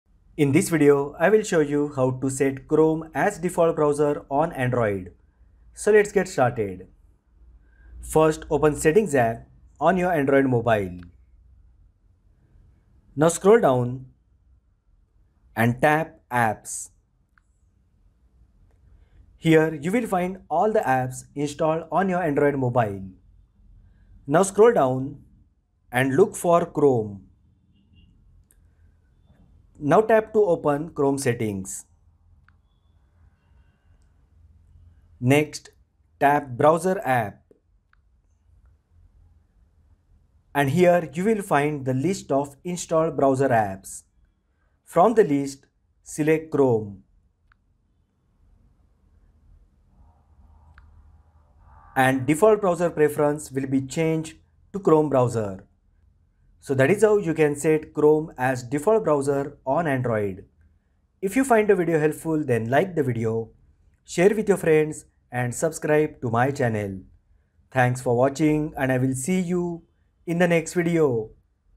How to Set Chrome as Default Browser on Android | Change Default Browser Android
How to Set Chrome as Default Browser on Android. Prefer using Google Chrome over other browsers on your Android device? In this video, we'll guide you through the process of setting Chrome as your default browser on Android. By making Chrome your default, all web links will automatically open in Chrome, giving you a consistent browsing experience. Follow our easy steps to switch your default browser settings and start enjoying seamless access to the web with Chrome on your Android device!